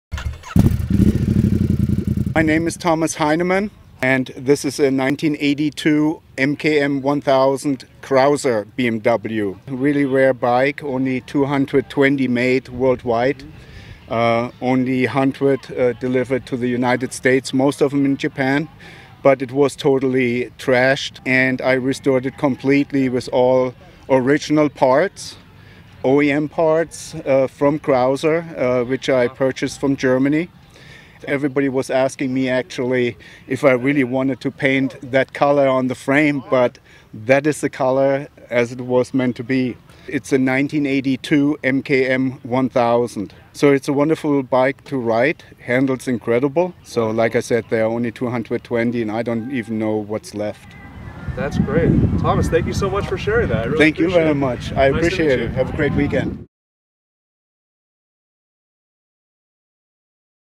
BMW Krauser MKM 1000 - Quail Motorcycle Gathering 2017
Thomas Heinemann shows us his beautiful Krauser MKM 1000.

Want to know more about this rare bike?  Here's a rehosted review from  a 1980 issue of Motorcyclist - though this review says that just 100 examples were built: